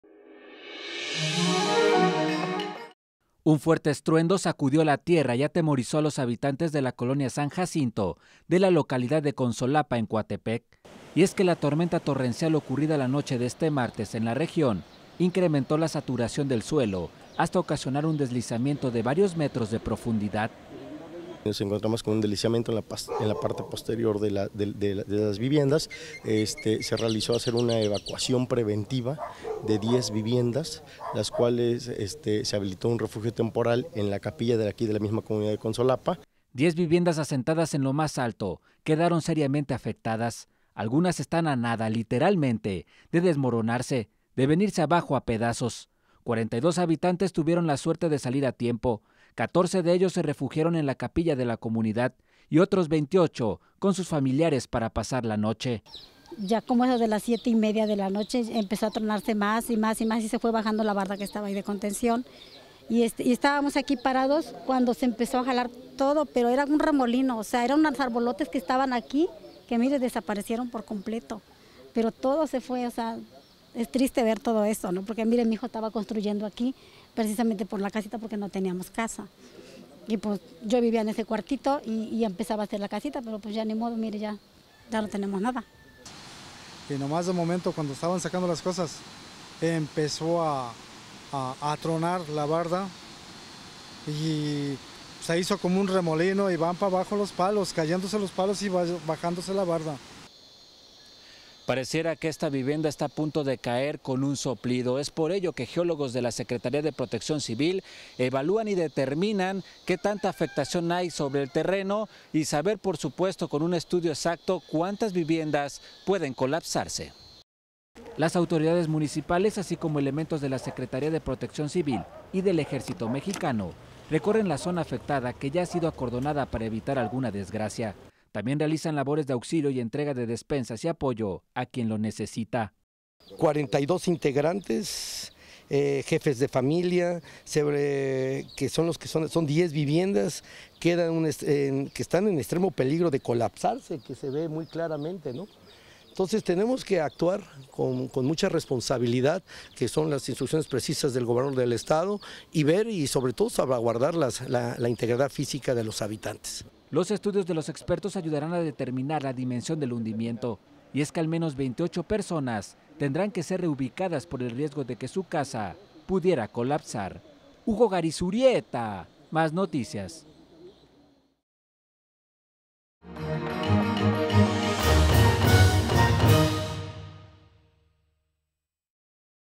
Lluvias dejan deslave en Consolapa
Un fuerte estruendo sacudió la tierra y atemorizó a los habitantes de la colonia San Jacinto de la localidad de Consolapa en Coatepec.

La tormenta  de la noche de este martes incrementó la saturación del suelo hasta ocasionar un deslizamiento de varios metros de profundidad.